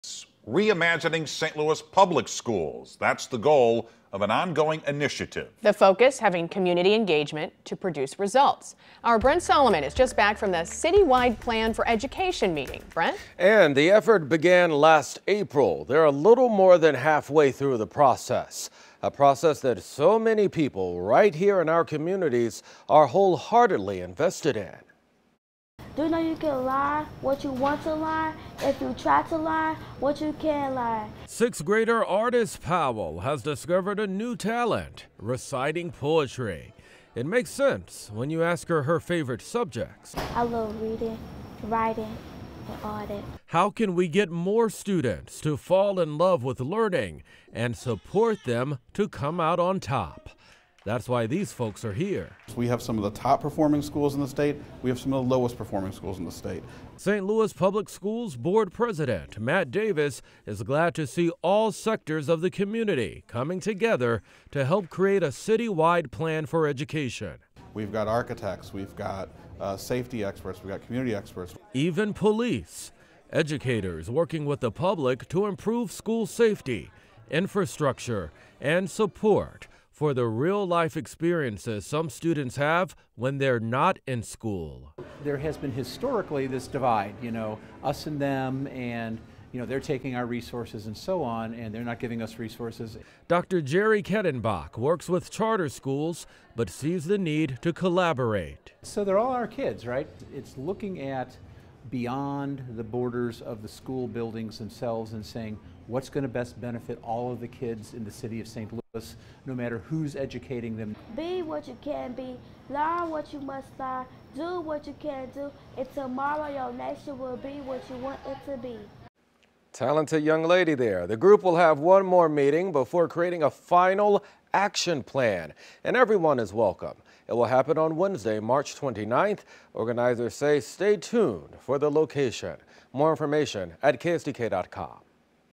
Reimagining St. Louis public schools discussed during citywide education plan meeting
The effort began last April. Our Brent Solomon provides the latest on what was discussed at the meeting.